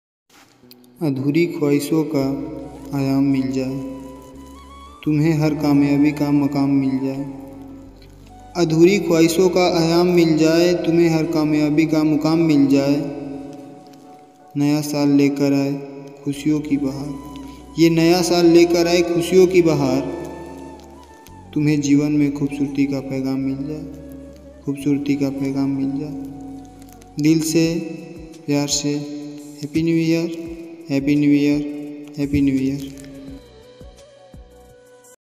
R name happy New year status 2022 (R नाम का हैपी न्यू इयर स्टेटस 2022) happy New year status shayari
Hi,
I am Anuj Tiwari. Welcome to our YOU tube channel at Colors Shayari status.
About this video- is video me R name ke liye new year wishes shayari hai jo ki aap apne whatsapp status ke liye use kar sakte hai.

Edit by- Anuj Tiwari

Voice- Anuj Tiwari

Advice- Sonu Tiwari